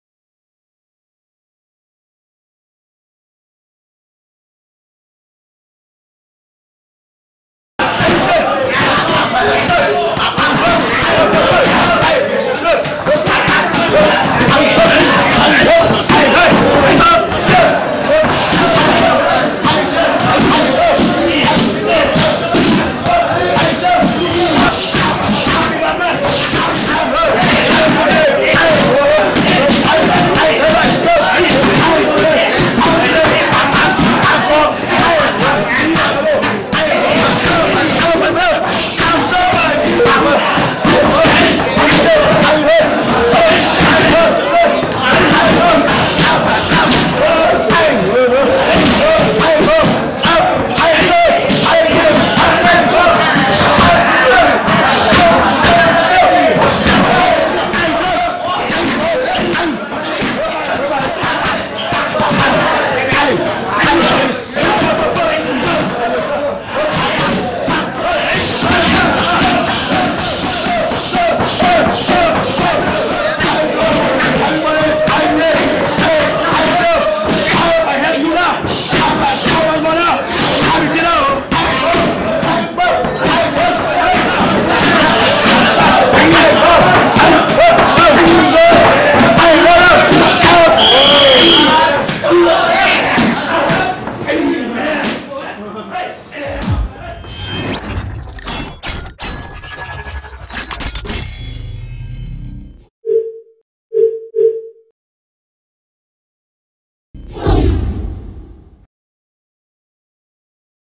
Pro Fighter Bruno "Robusto" Miranda Fast Pad Work
Tiger Muay Thai fight team member, Brazilian MMA and Kickboxer,  Bruno "Robusto" Miranda does pad work before an upcoming fight.
The 24 year-old Miranda has been fighting professionally since he was 16, amassing over 40 fights combined in Muay Thai, MMA, K-1 and boxing.